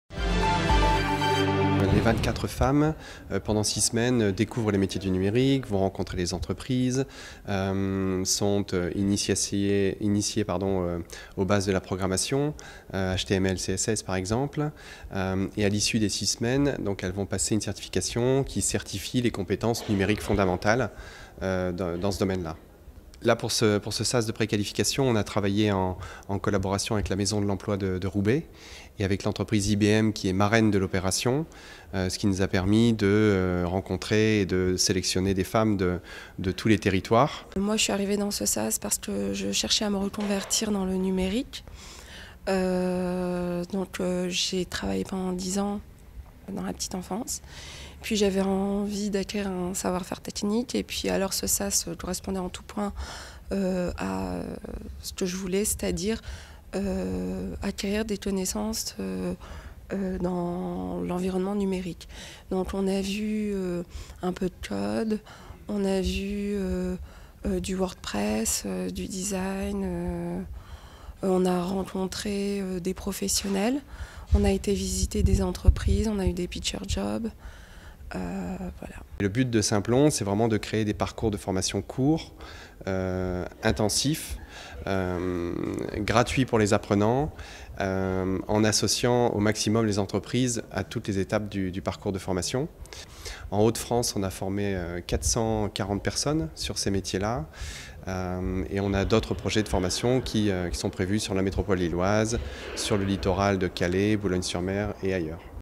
Une formation "hackeuse" pour découvrir les métiers de l'informatique
La Start up Simplon existe depuis 6 ans. Et elle dispense une formation « hackeuse » pendant 6 semaines.

Cette formation, c'est un sas de pré-qualification à destination de 24 femmes intéressées par les métiers du numérique mais qui n’ont pas de projet consolidé dans ce secteur. La formation vise à aborder/découvrir l’ensemble des métiers du numérique pour consolider ensuite un projet dans ce domaine.